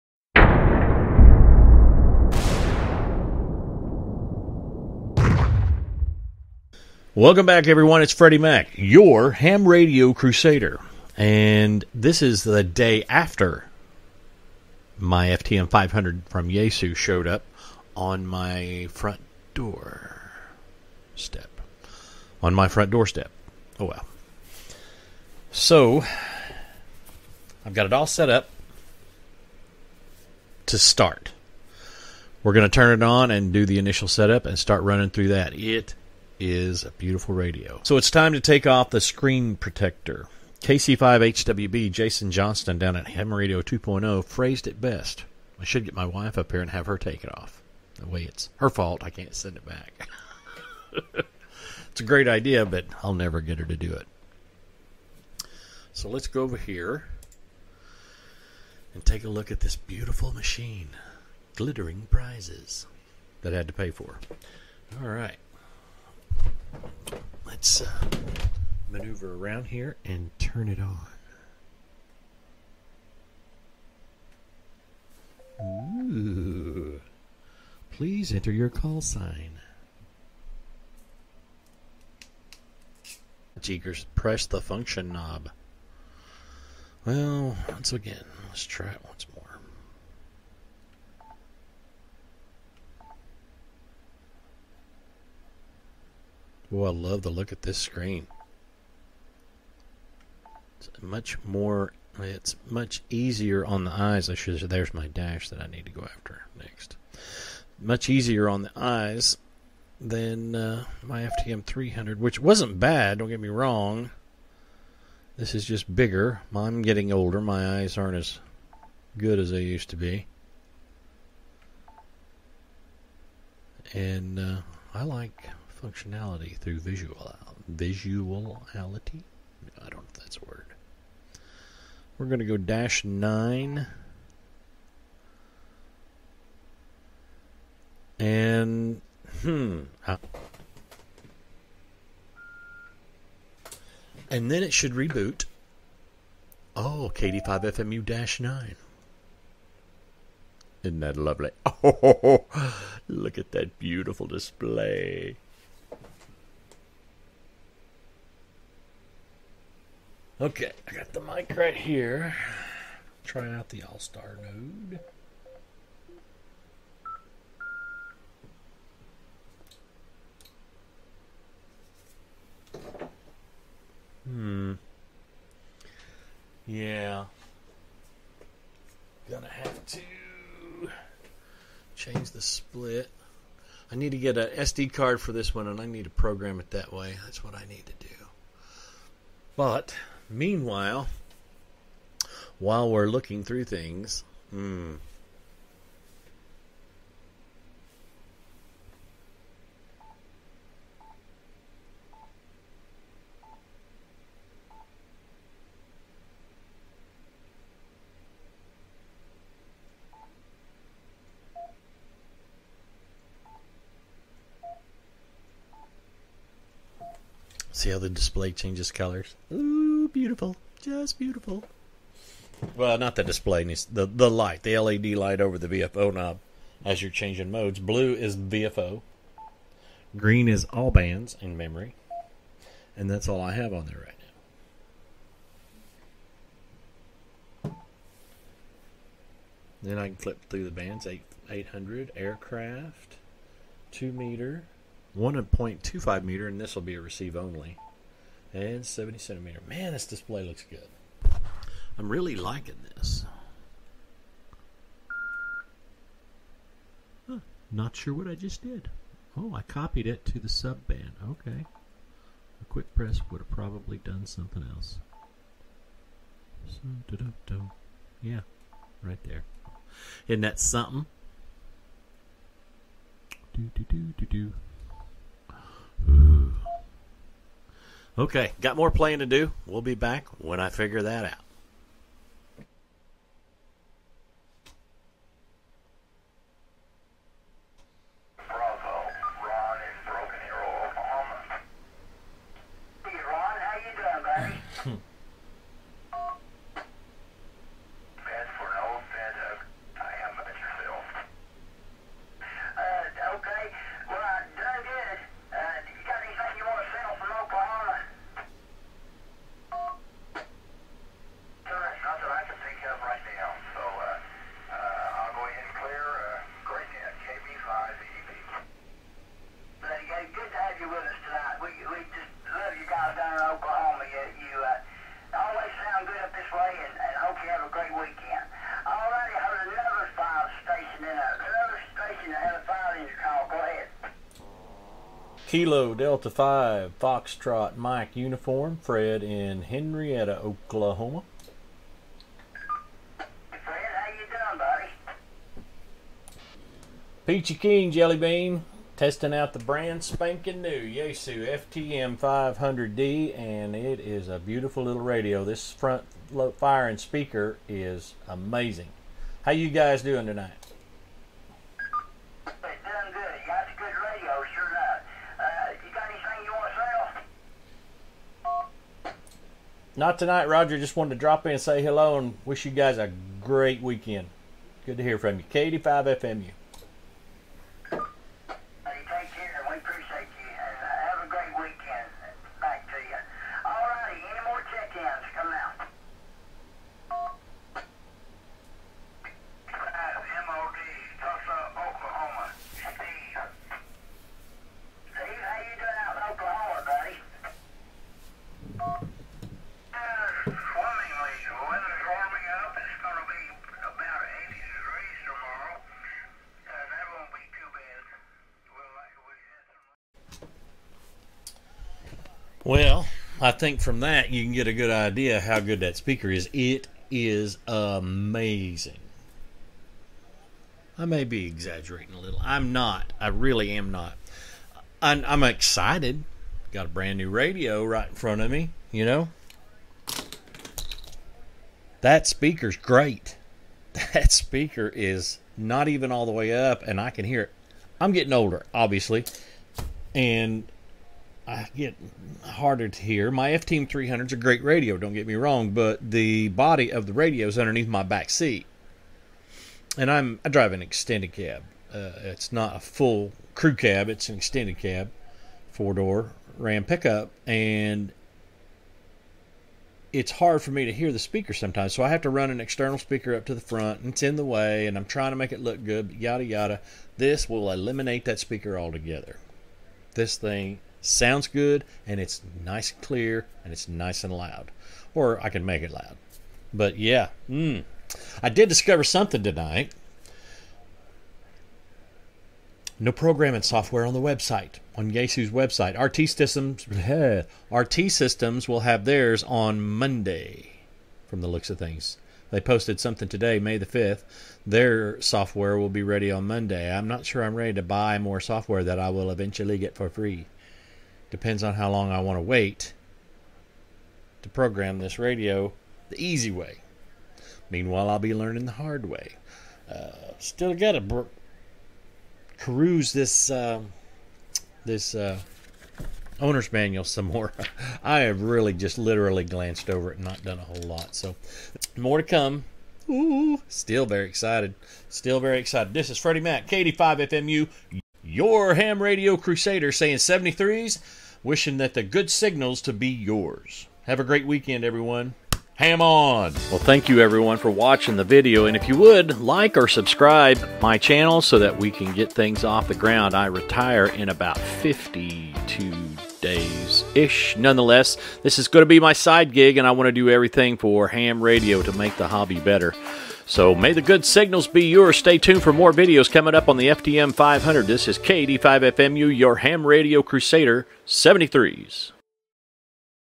FTM 500 Part 2 Initial Setup and Analog Audio Test
Hello Crusaders!! This is Part 2 to the Yaesu FTM-500DR Radio Series and today we do the Initial Setup and Analog Audio test. This speaker on the control head is AWESOMENESS!!! Hope you enjoy the video.

73 DE KD5FMU Your Ham Radio Crusader!  HAM ON!!!

Check us out on TikTok and Instagram, @OkieArchaeology

God Bless and God Love Ya!!